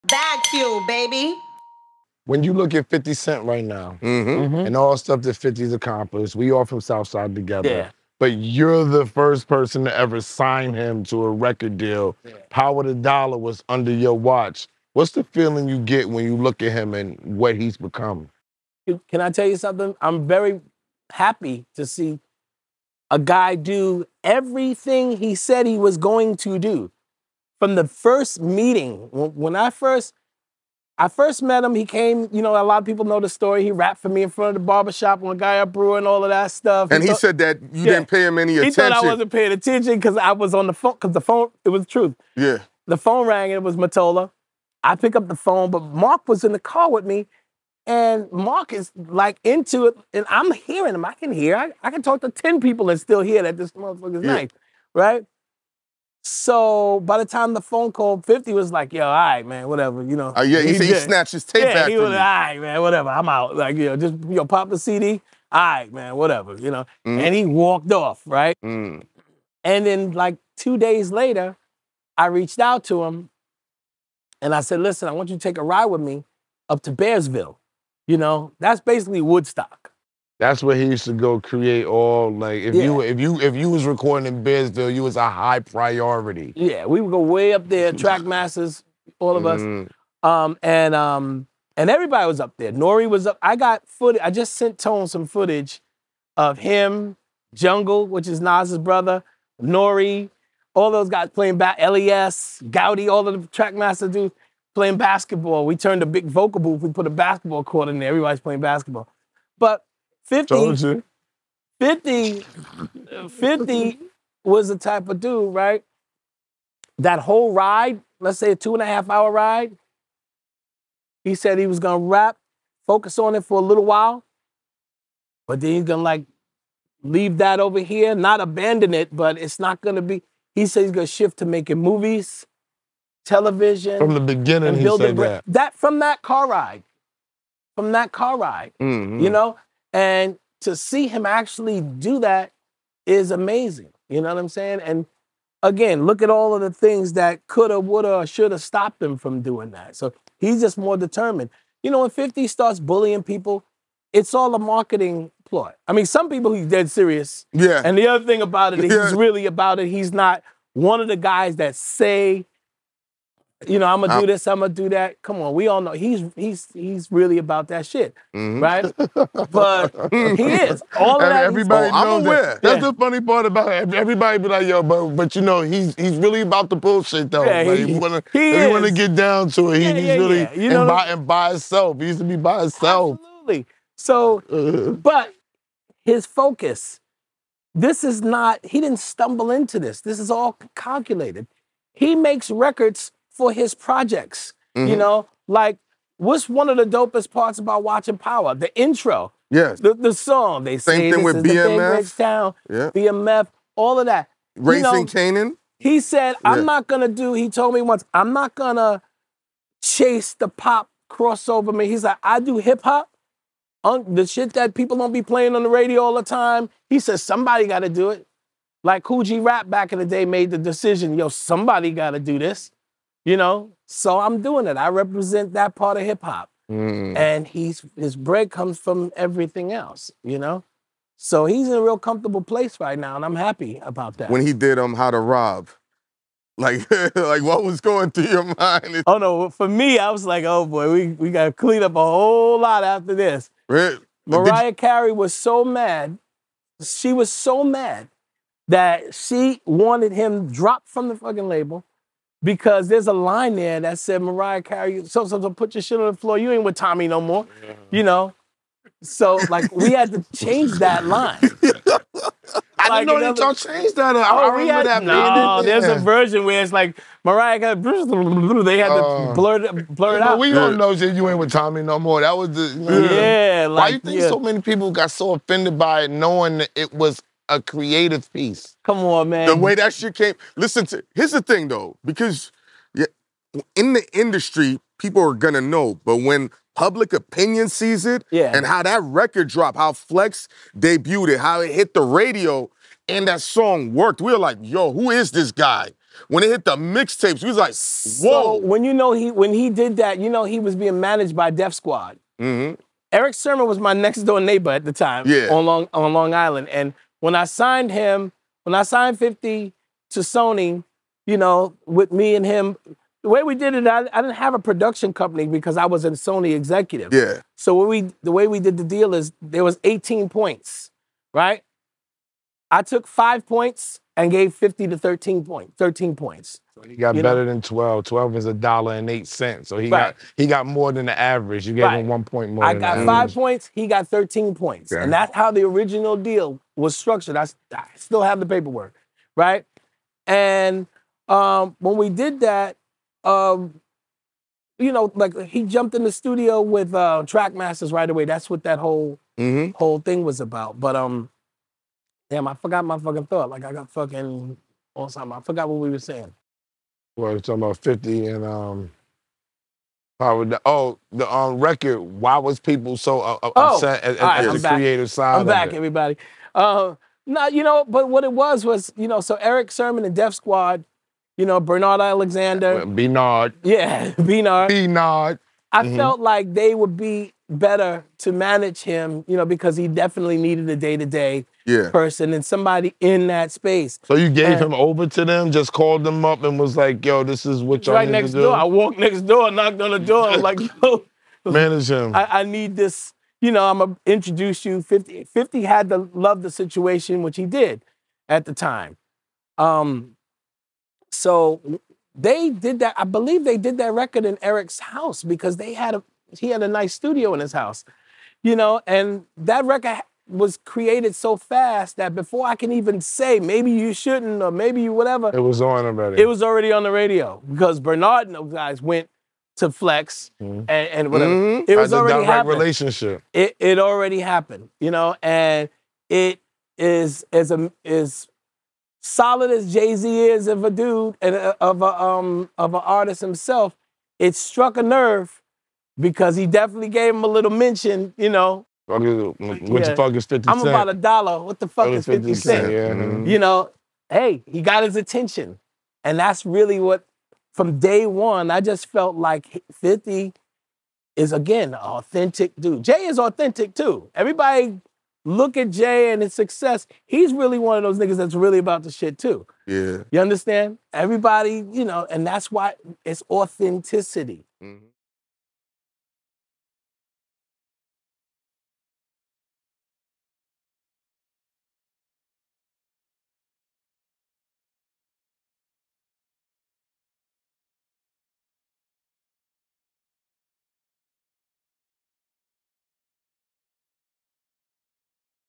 Cory Rooney Expresses His Feelings On 50 Cent's Success
Cory Rooney breaks down how he met 50 Cent and what he told him he was going to achieve in his career.